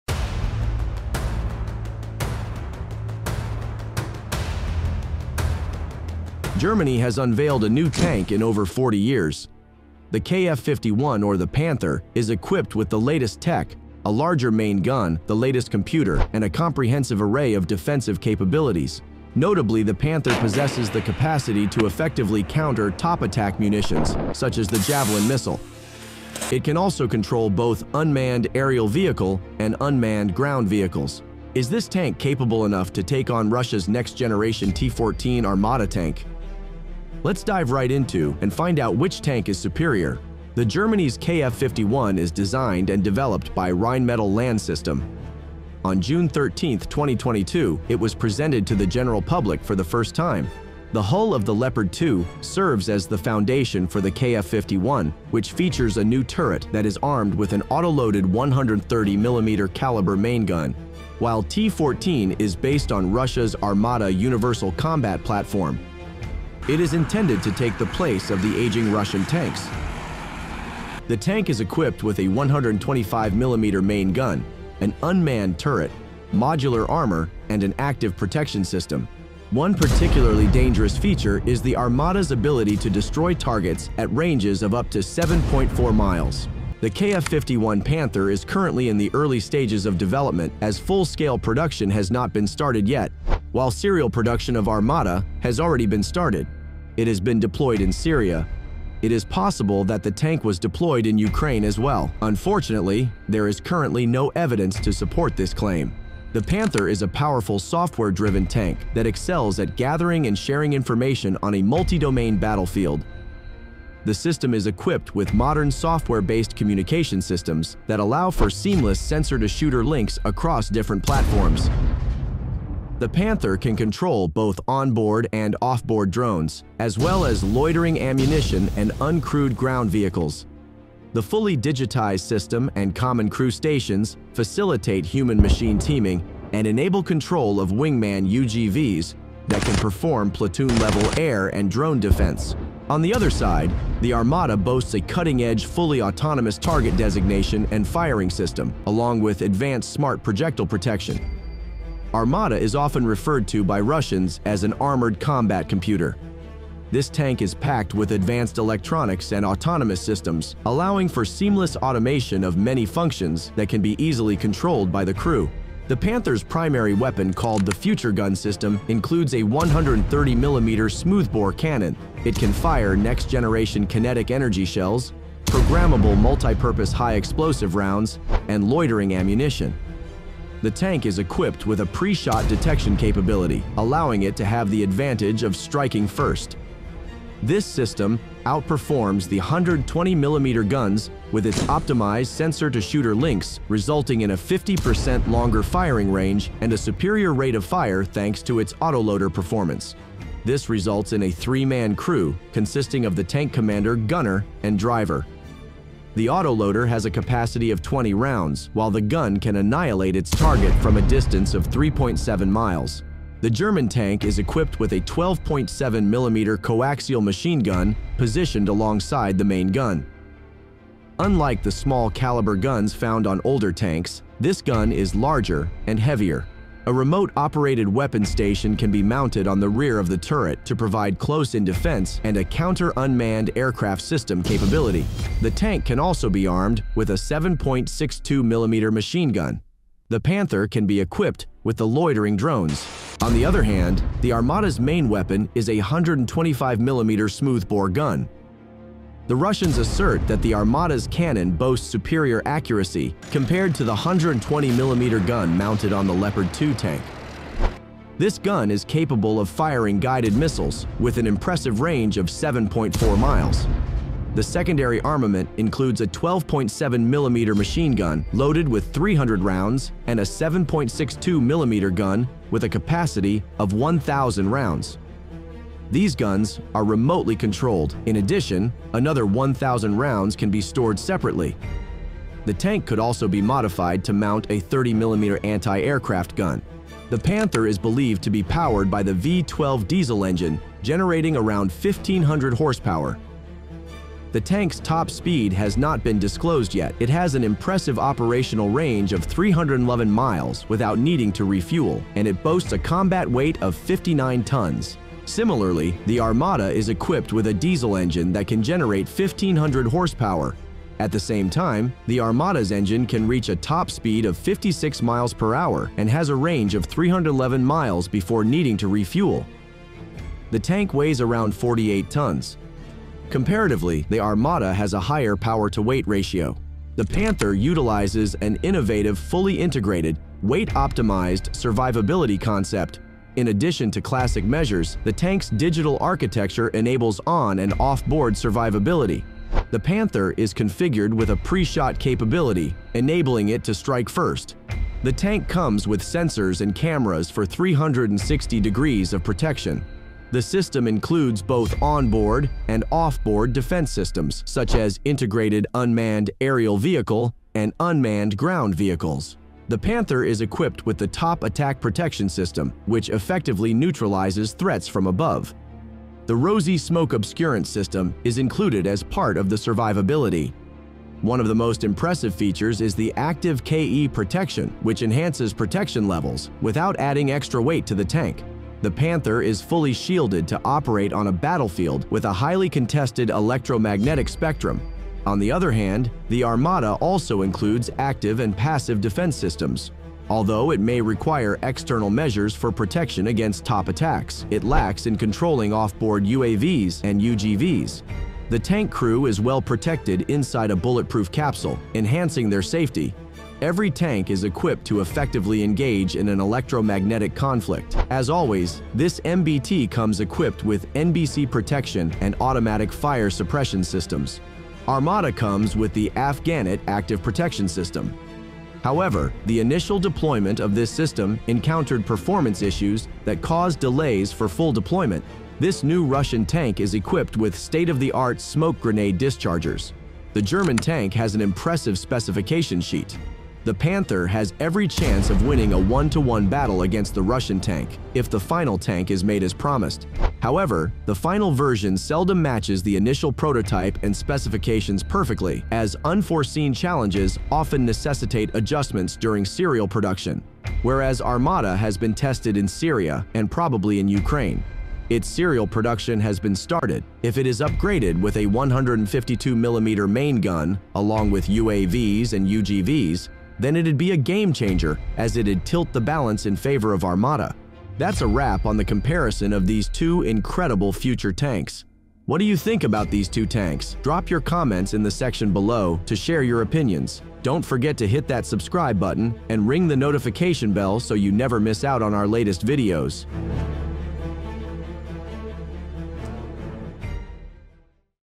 KF51 Panther vs T-14 Armata - which tank is better?
Germany has unveiled a new tank in over 40 years --- The KF51 or the Panther, is equipped with the latest tech, a larger main gun, the latest computer, and a comprehensive array of defensive capabilities -- Notably the Panther possesses the capacity to effectively counter top attack munitions, such as the Javelin missile. It can also control both Unmanned Aerial Vehicle and Unmanned Ground Vehicles. Is this tank capable enough to take on Russia’s next-generation T-14 Armata tank? Let’s dive right into and find out which tank is superior.

The Germany’s KF51 is designed and developed by Rheinmetall Land system. On June 13, 2022, it was presented to the general public for the first time. While T-14 is based on Russia’s Armata Universal Combat Platform. The T-14 is intended to take the place of the aging Russian tanks. The Panther is a powerful software-driven tank that excels at gathering and sharing information on a multi-domain battlefield. On the other side, the Armata boasts a cutting-edge, fully autonomous target designation and firing system, along with advanced smart projectile protection. The panther's primary weapon called the Future Gun System, includes a 130-millimeter smoothbore cannon. This system outperforms the 120-millimeter guns with its optimized sensor-to-shooter links resulting in a 50% longer firing range and a superior rate of fire thanks to its autoloader performance. The German tank is equipped with a 12.7 millimeter coaxial machine gun positioned alongside the main gun. The tank can also be armed with a 7.62-millimeter machine gun. The Panther can be equipped with the loitering drones. On the other hand the Armata's main weapon is a 125-millimeter smoothbore gun. This gun is capable of firing guided missiles, with an impressive range of 7.4 miles. The secondary armament includes a 12.7-millimeter machine gun loaded with 300 rounds and a 7.62-millimeter gun with a capacity of 1,000 rounds. The Panther is believed to utilize the V12 diesel engine, generating around 1500 horsepower. Similarly, the Armata is equipped with diesel engine which can generate 1500 horsepower.
For category wise comparison and detailed conclusion, watch the complete video.

Intro                 00:00
OVERVIEW         00:40
Digitization         01:46
Weapon Systems 02:45
Engine                 04:46
Survivability         05:26
Conclusion         07:10

Credits
1) Bundeswehr
2) Минобороны России
All content on Grid 88 is presented for only educational purposes. The appearance of US DOD doesn’t imply or constitute the respective entities or this channel’s endorsement.